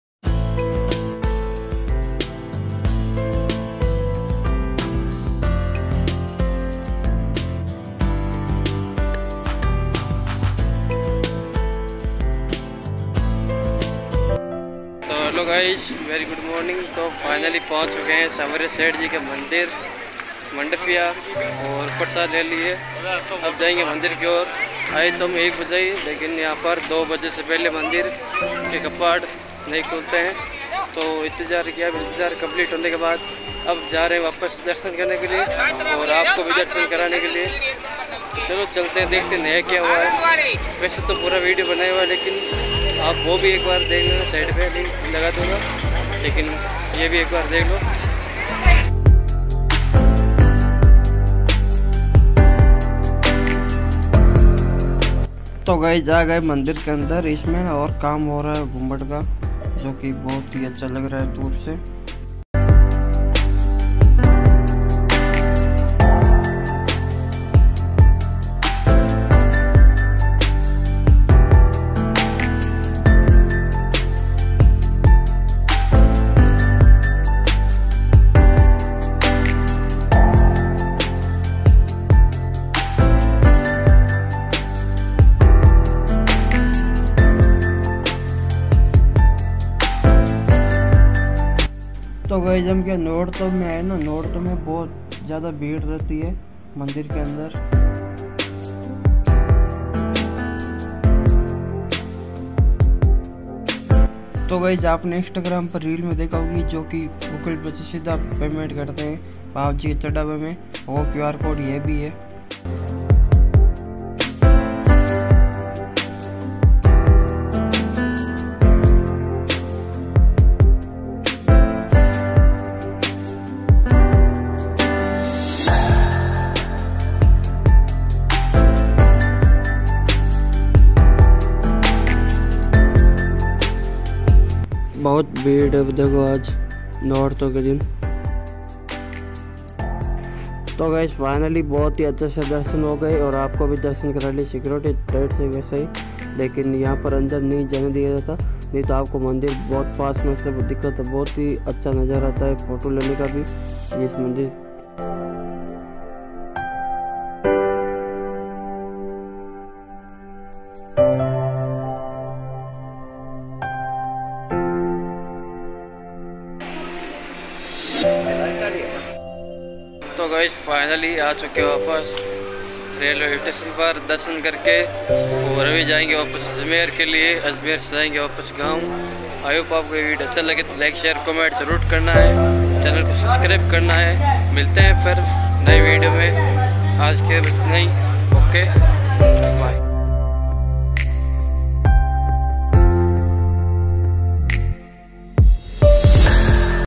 सांवलिया सेठ जी के दर्शन करते वक्त इस सोने से बने पुरे मंदिर को देखकर हम दोनों आश्चर्यचकित रह गए 🧐
Sanwaliya Seth Temple Ke Darshan kar liye 😍🙏 Mandfiya Chittorgarh Vlog

||{Dinesh vlog 0001}|| \\
 {सब रियल है}